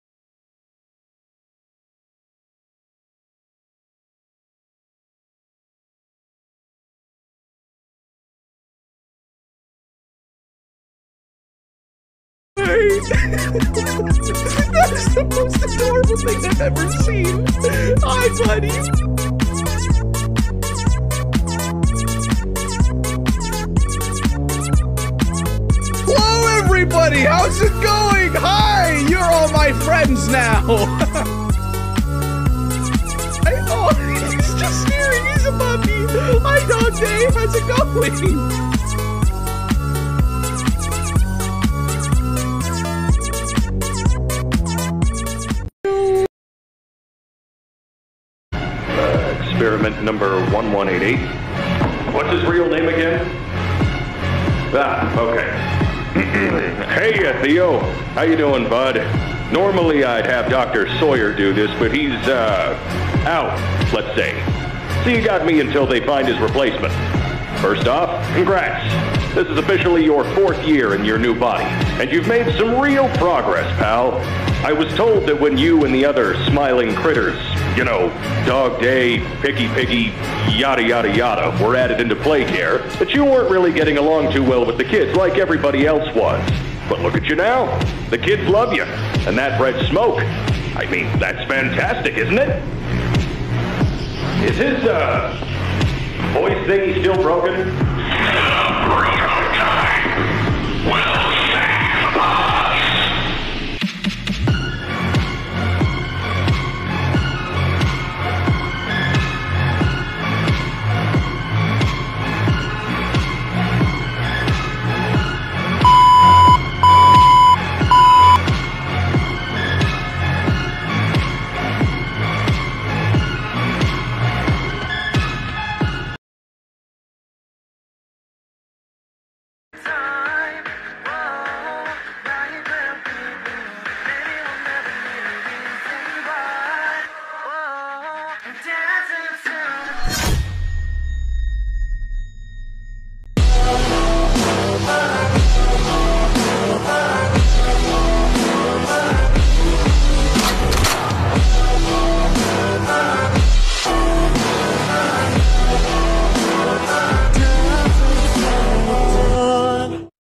The smiling critters react to Dogday & Catnap (Bad quality-)
Memes/Videos belong to: @LNOXEDITZ on TikTok & @Jade on TikTik

reaction made by: Me

Apps used: Gacha Club & CapCut

Inspired by: @Ayavensu

Enjoy! I'm so sorry abt the bad quality :( I might post it on Instagram soon.